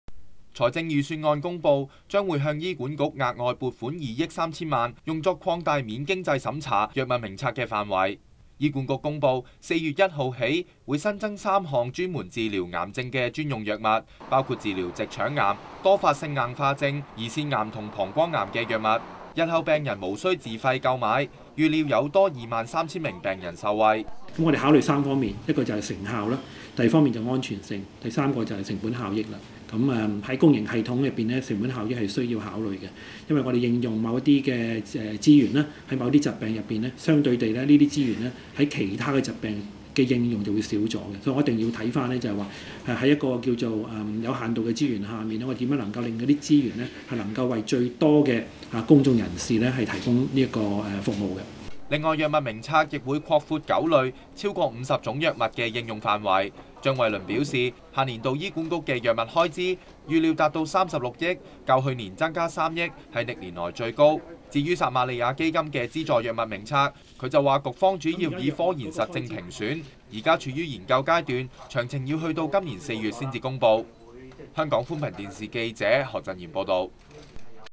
新增三項癌症藥毋須自費
醫管局四月起，將治療直腸癌、胰腺癌、膀胱癌和多發性硬化症的三項專用藥物，加入藥物名冊，日後病人毋須自費購買，預料會有多23000名病人受惠。